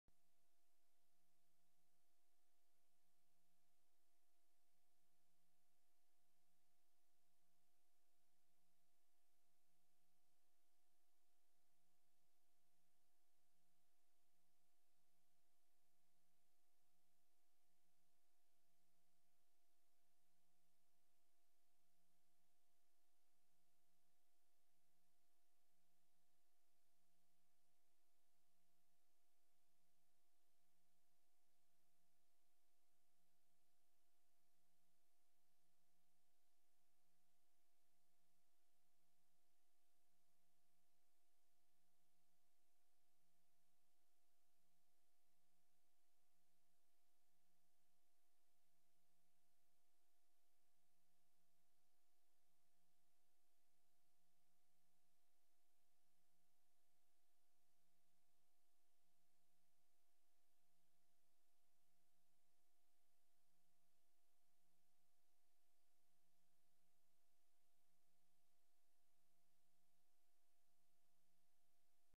KING - NO SOUND
Airport building. Control tower. Nehru and President Radhakrishna approach plane. King Savanng. Prince Souvanna Phouma leave plane. Greeted. Delhi Mayor garlands King. Flags. King inspects Guard of Honour. Bodyguard patrol car.